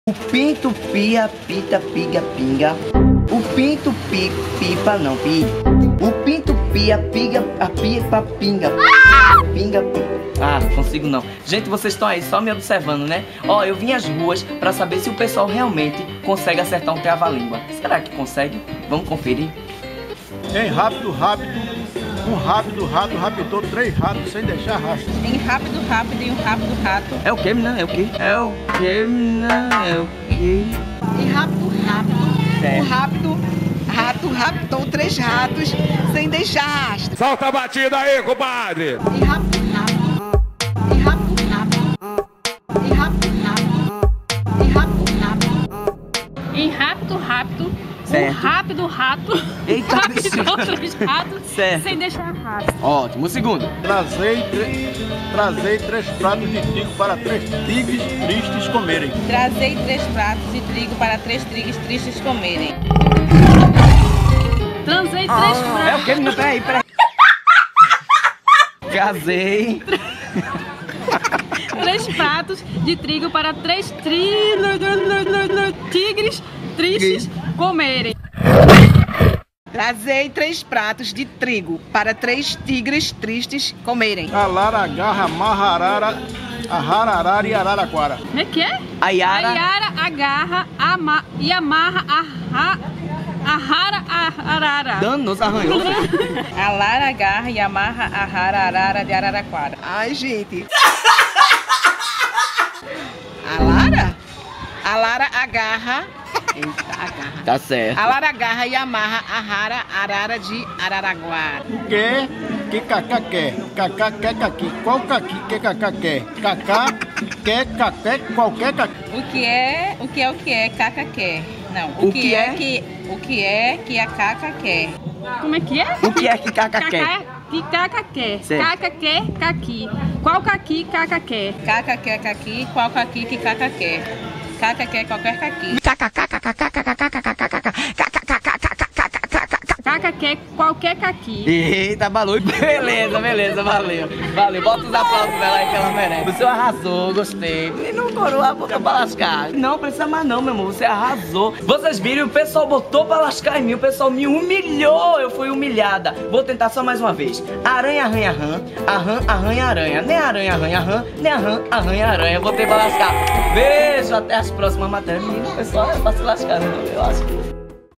Trava Lingua | Betty Xuca nas ruas
Betty Xuca vai as ruas pra testar o trava lingua com as pessoas.